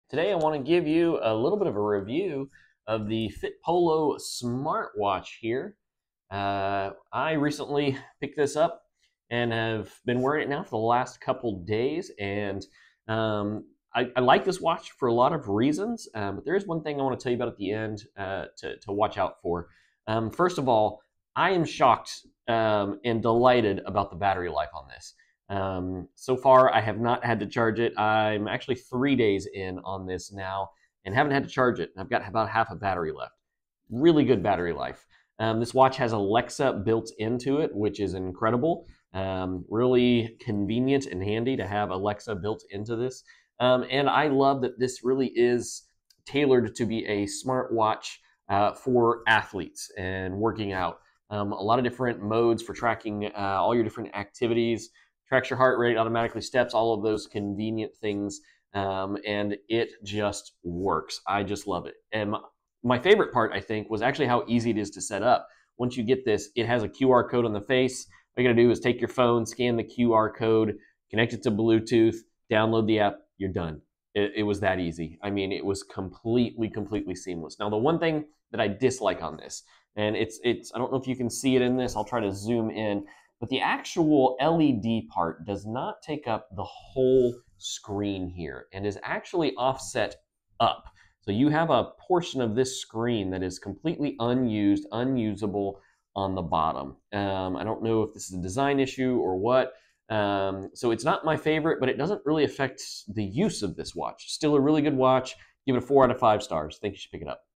Fitpolo Smart Watch Review: Alexa built-in fitness tracker
Review of Fitpolo Smart Watches for Men/Women, 1.8'' Alexa Built-in Fitness Tracker Watch with Bluetooth Calls, IP68 Waterproof, Heart Rate/Sleep/SpO2/Stress Monitor, 110 Sport Modes for Android & iPhone